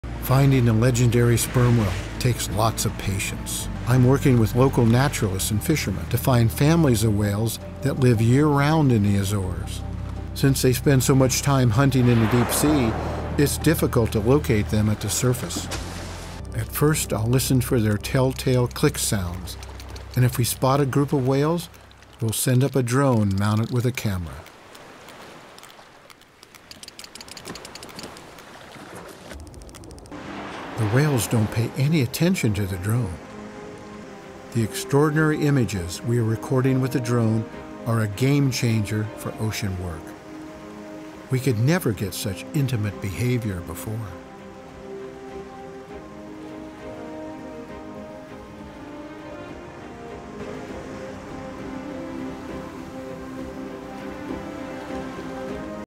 Perfect Shot: Filming Sperm Whales 🐳 Living in the Azores for Wildlife Documentary
Follow legendary filmmaker Rick Rosenthal on the journey to film sperm whales 🐳. Sperm Whales live all year round off the coast of the Azores. They spend most of their time in the deep sea hunting for giant squid. To find them you need a good microphone and listen to their characteristic click sounds.

If you are lucky, you can find them resting at the oceans surface, enjoying the sun and family time. Only with the help of the locals and with enough skill, such beautiful shots are possible.